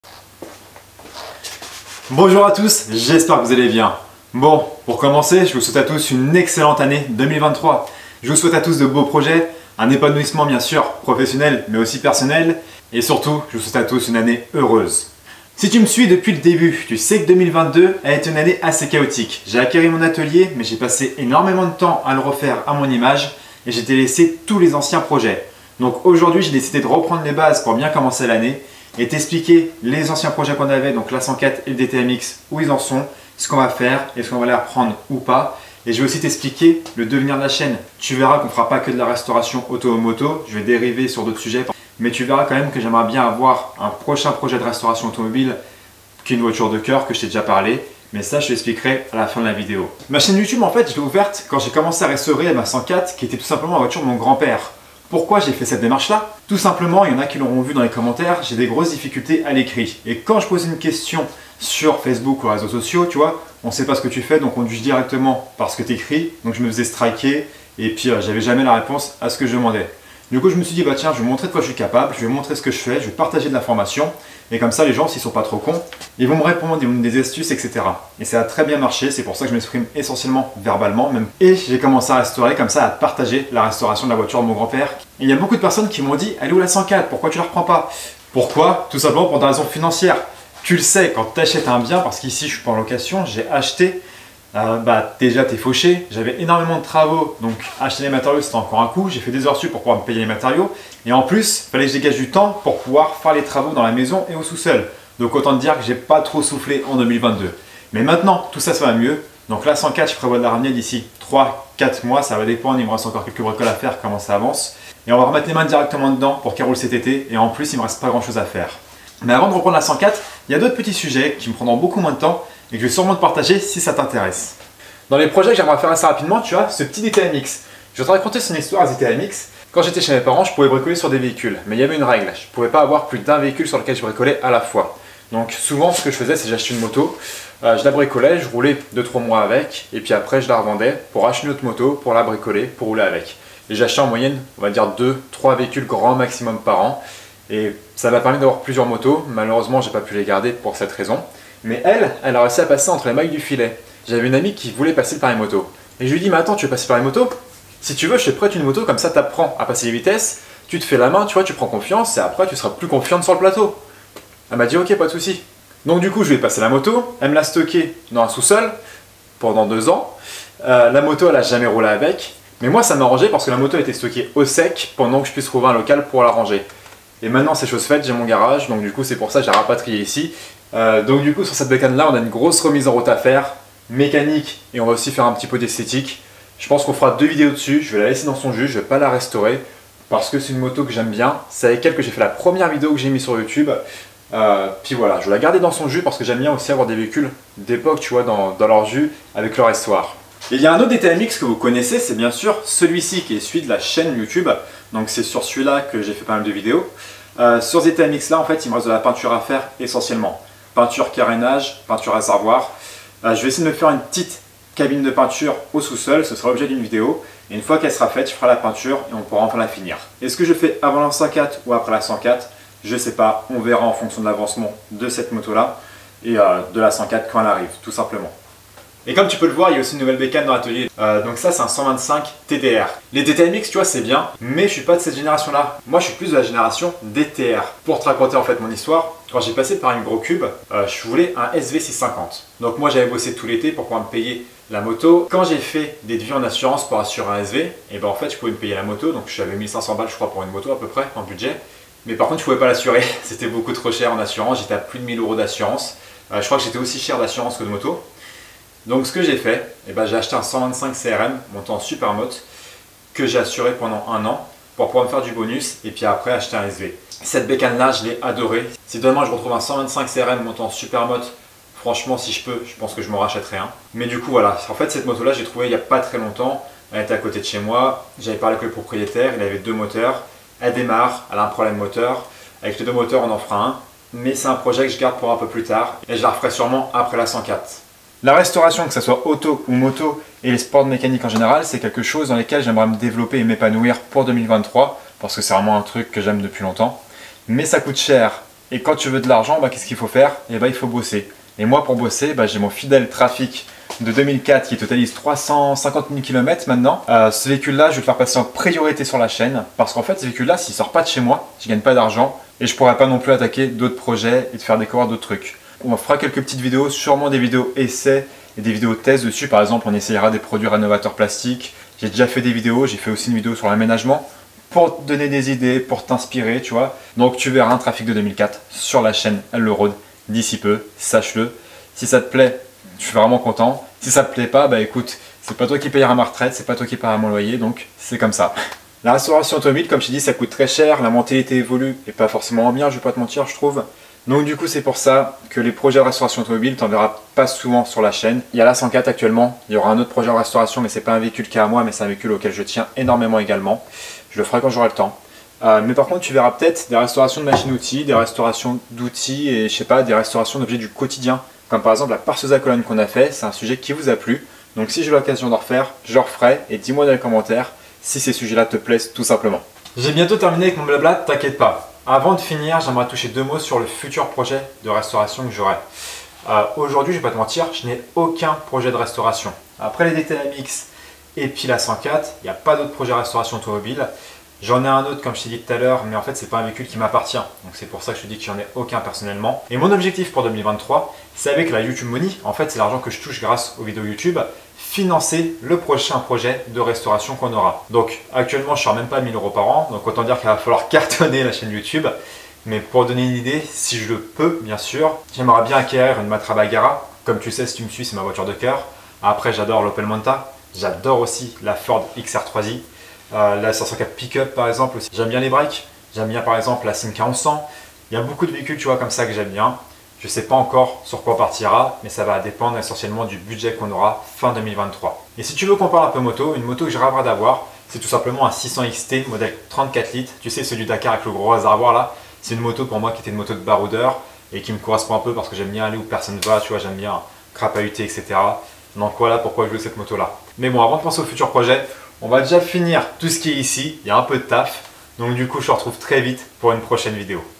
On reprends tous les projets ! Point sur les objectifs de 2023 ;)
Pour bien commencer l'année, j'ai décider de reprendre les base et de faire un point avec toi sur les projet qui ce réaliserons en 2023. au programme de la restauration auto, moto mais aussi d'objet et d'outils du quotidien ;)

📱 Facebook : (groupe d'aide pour la restauration auto et moto basé sur l’altruisme et l’échange) ▶︎